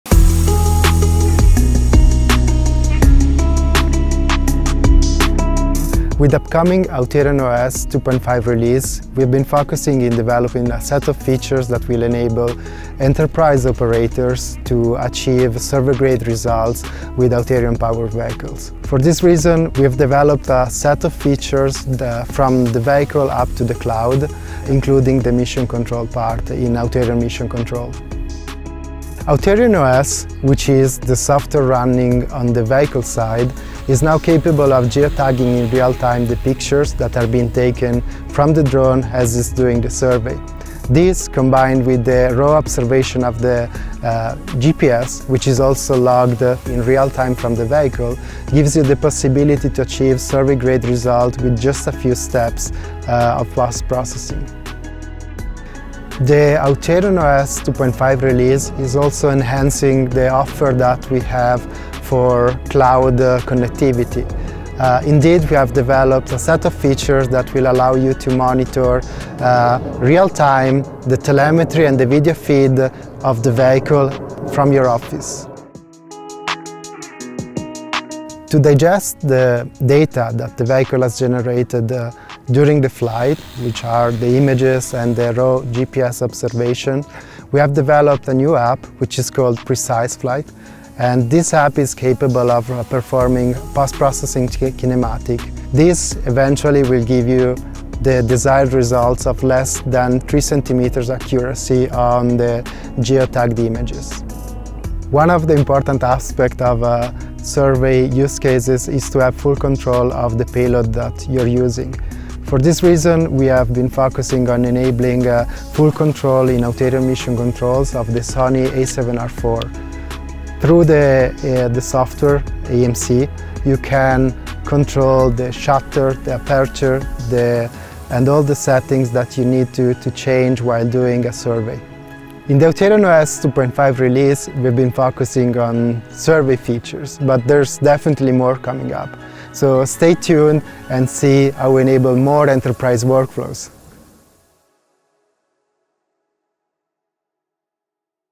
High precision mapping workflows with AuterionOS 2.5
With its latest software release, Auterion introduces new capabilities for high precision mapping missions and automated end-to-end workflows. Watch this video interview with our engineering manager Claudio Micheli to find out more.